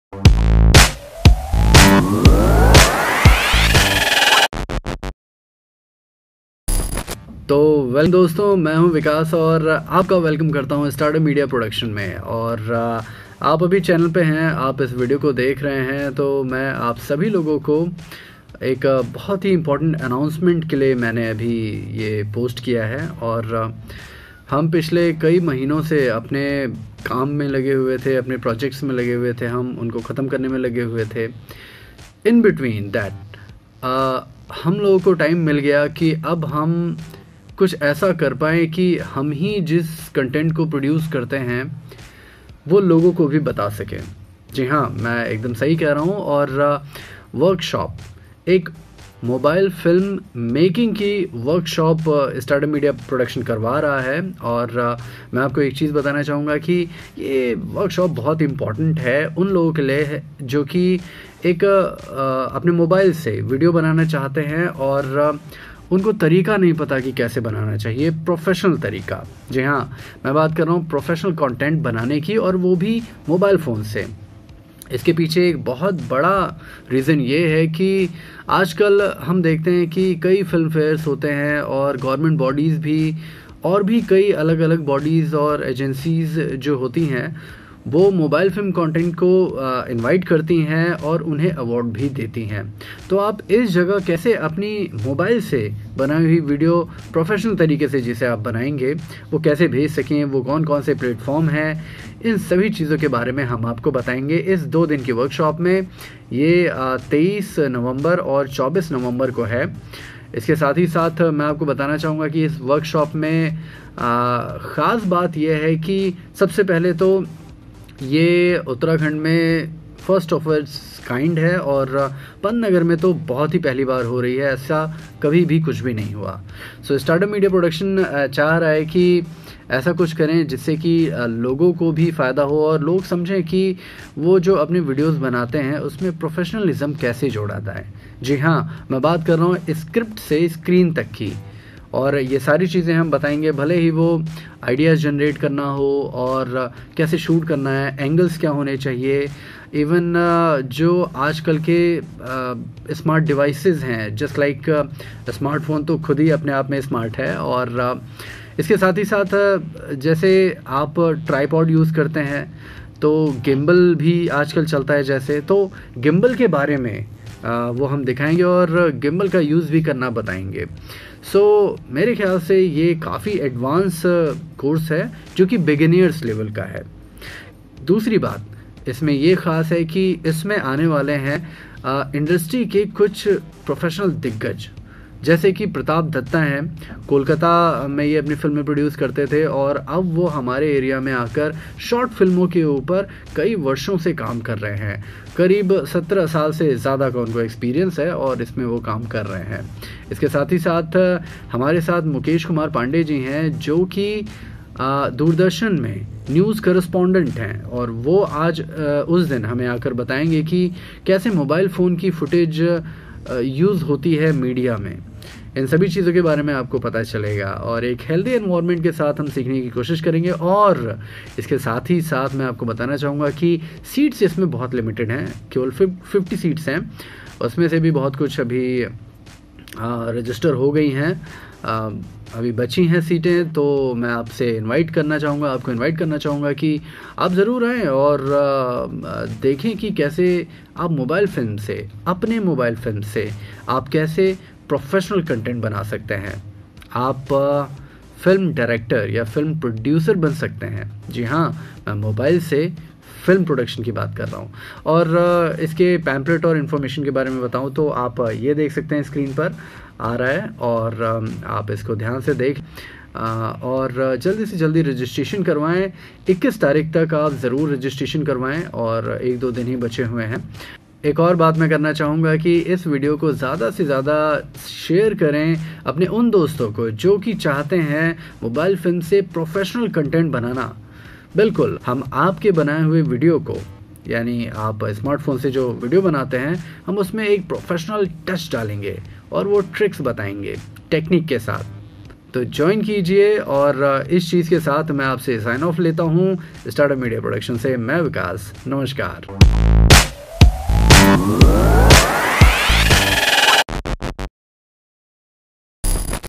Announcement workshop date 23to 24 Nov 2019
We welcome you all film lovers and video content creators, in this workshop we make you learn art of video creation by your phone.